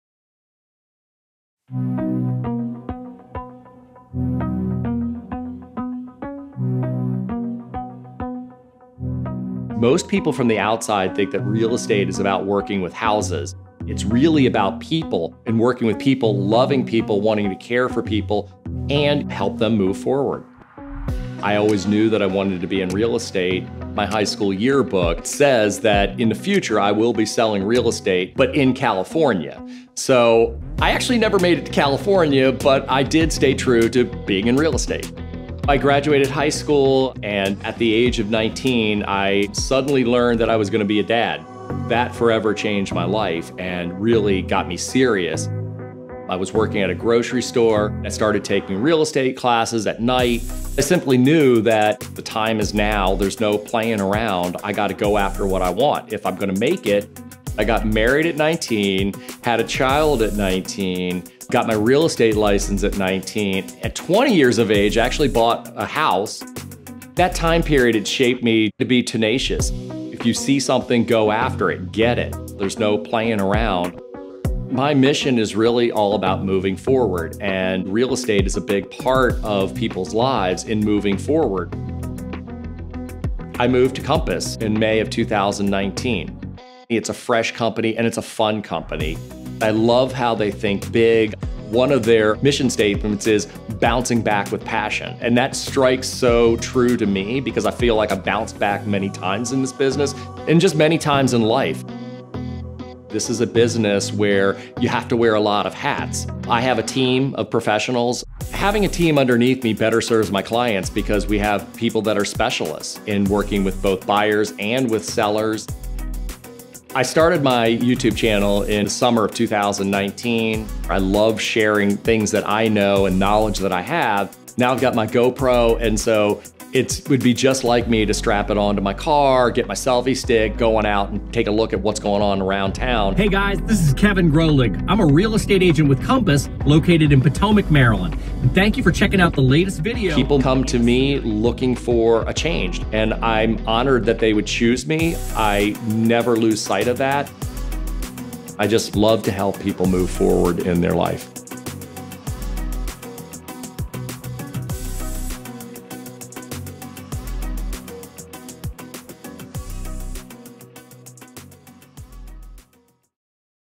Who is Kevin Grolig? Real Estate Agent in Montgomery County
Hi, I am Kevin Grolig - Real Estate Agent here in Montgomery County, Maryland. If you are looking to sell your home, or purchase a new home in the Maryland area I am here to make your real estate transaction go as smooth as possible.